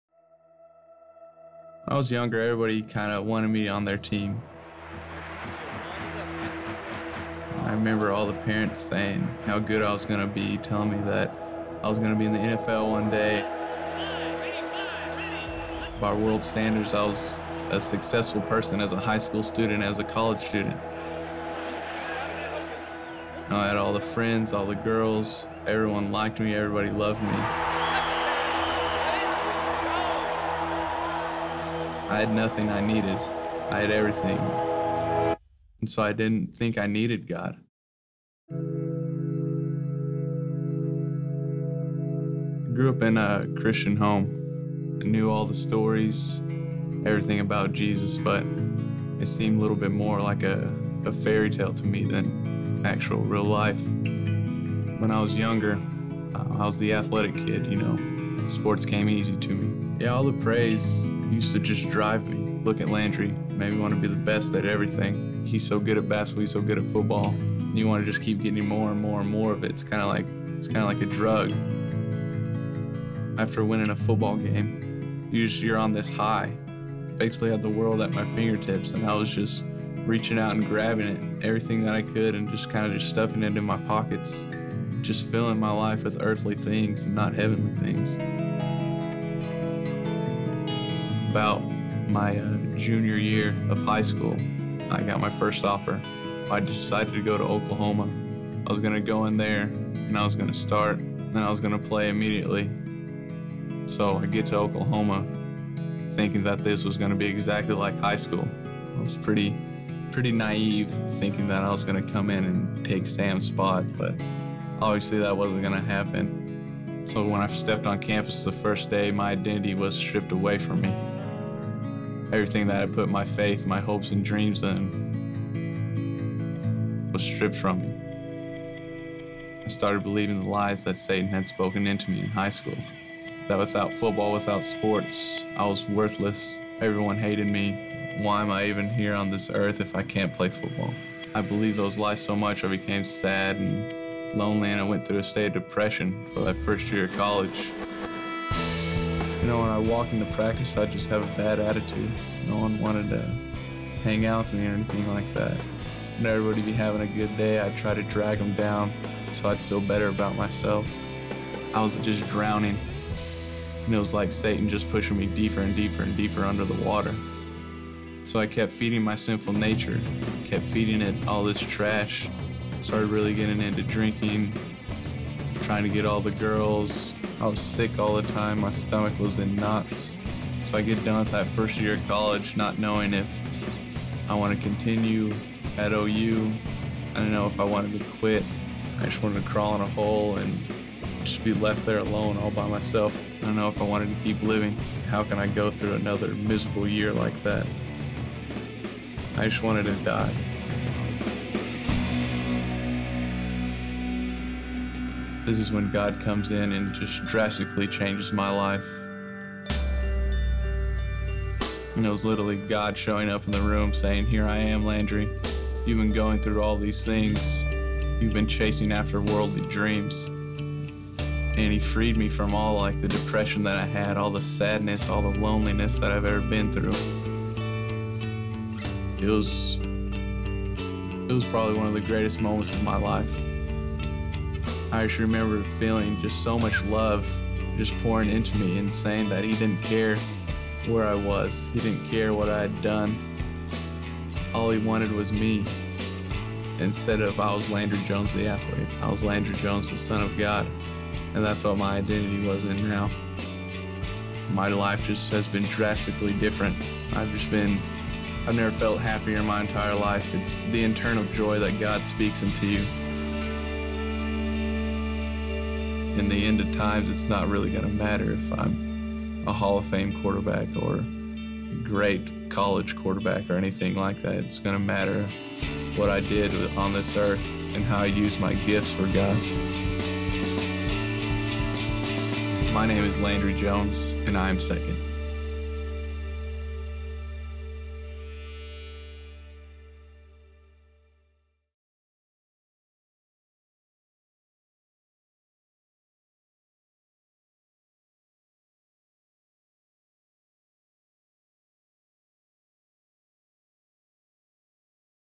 Landry Jones - White Chair Film - I Am Second®
Sports always came easy for Landry Jones. Everyone wanted him for their team. Everybody loved him. He had friends, girls-- the world was at his fingertips. Yet he still desired more.

Playing football at the University of Oklahoma was an easy decision to make—arriving on campus and discovering he was low man on the depth chart was another. He was no longer the star, no longer desired by everyone. After all, Oklahoma had the Heisman Trophy winner, Sam Bradford on their team. Landry was out of the spotlight.

Having his identity stripped away was a shock to Jones. He didn't know how to handle it which showed in a bad attitude and his pursuit of satisfaction in other things. But then came an encounter with God that changed his attitude, his focus, and ultimately his life.

He now has a new identity, one given to him by God.